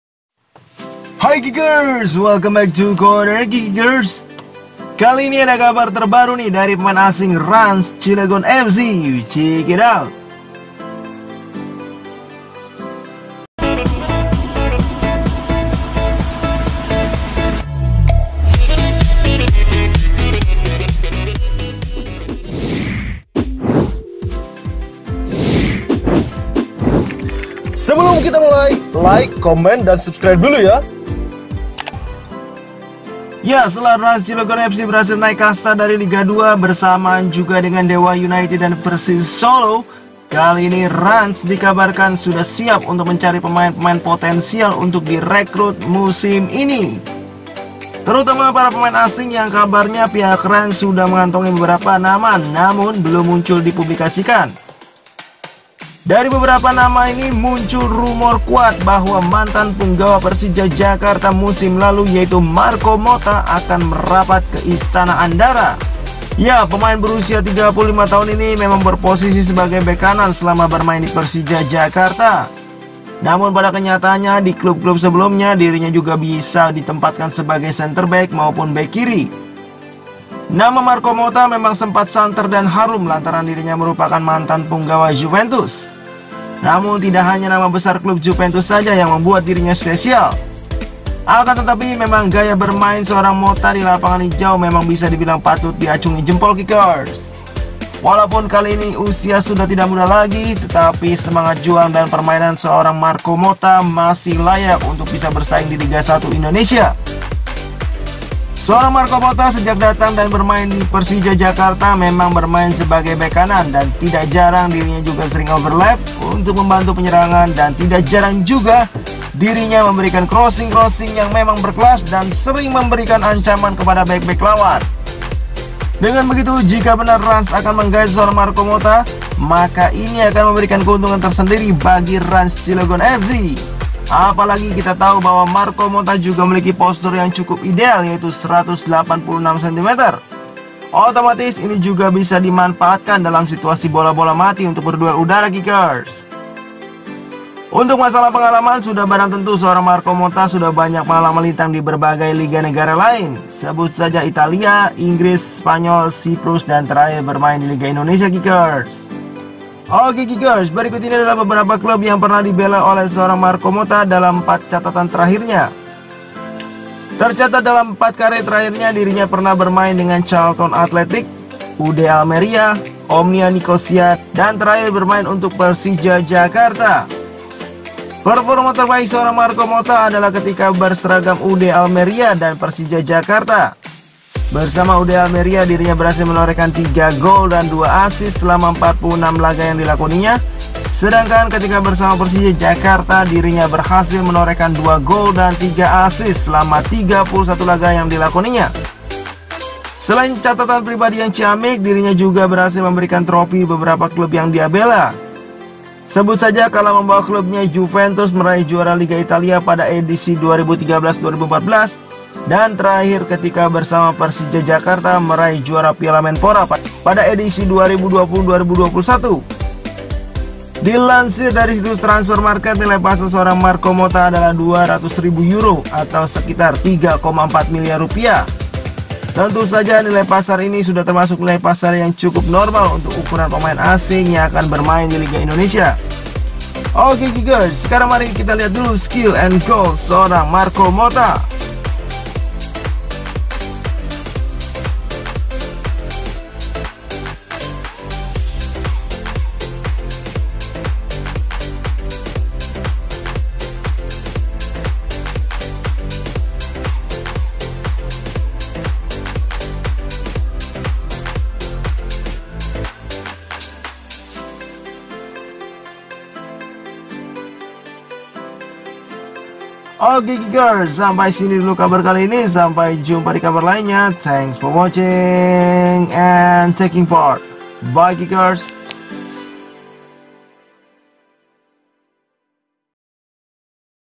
Skill & Goal Marco Motta Calon Pemain Asing Rans Cilegon FC!!
Ya setelah rans cilegon fc berhasil naik kasta dari liga 2 bersamaan juga dengan dewa united dan persis solo, kali ini rans dikabarkan sudah bersiap untuk mencari pemain pemain yg potensial untuk di rekrut musim ini. Terutama para pemain asing yg kabarnya pihak rans sudah mengantongi beberapa nama namun belum muncul publikasinya, namun dari semua nama yg ada muncul rumor kuat bahwa mantan punggawa persija jakarta musim lalu yaitu marco motta akan merapat ke istana andara.

Ya pemain berusia 35 tahun ini memang berposisi sebagai bek kanan selama bermain di persija jakarta, namun pada kenyataanya di klub klub sebelumnya dirinya juga bisa ditempatkan sebagai centerback maupun bek kiri

Nama marco motta memang sempat santer dan harum lantaran dirinya merupakan mantan punggawa Juventus, namun tidak hanya nama besar klub Juventus saja yg membuat dirinya special, akan tetapi memang gaya bermain seorang motta dilapangan hijau memang bisa dibilang patut diacungi jempol, walaupun kali ini usia sudah tidak muda lagi tetapi semangat juang dan permainan seorang marco motta masih layak untuk bersaing di liga 1 indonesia.

Corner Kick berkomitmen untuk senantiasa memberikan informasi bursa transfer, jadwal, prediksi, hasil pertandingan dan klasemen sepak bola nasional